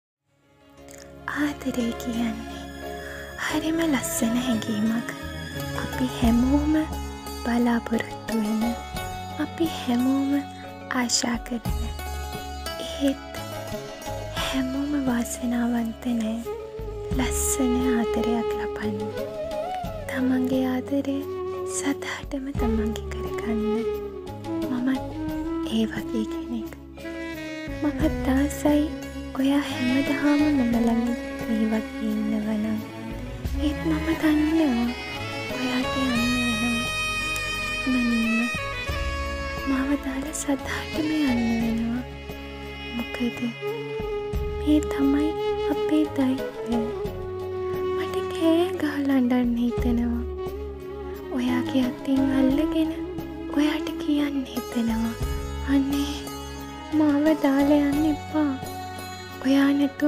Sinhala (Srilankan language) female voiceover sample - Madhushika W.
Sinhala (Srilankan language) female voiceover talent Madhushika W. is ready to voice your next upcoming video's Sinhala version in India.